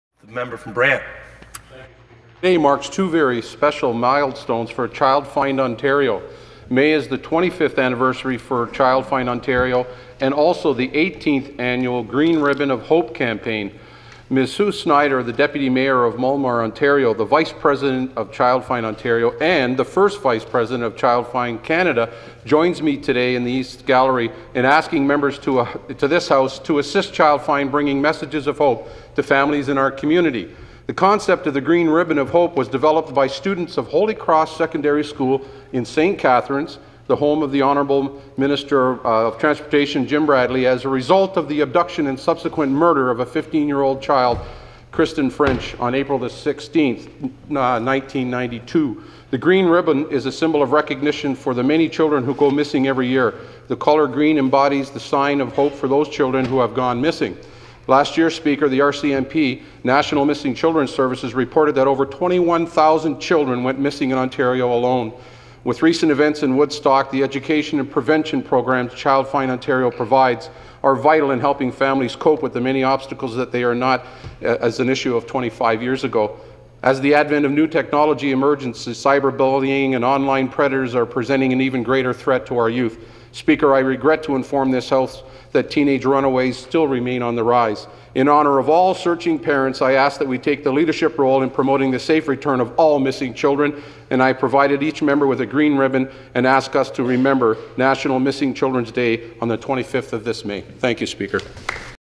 Dave Levac Statement re Child Find Green Ribbon Campaign 05 25 09
Dave Levac's Member Statement to the Legislative Assembly of Ontario regarding "Child Find Green Ribbon Campaign" on May 25, 2009.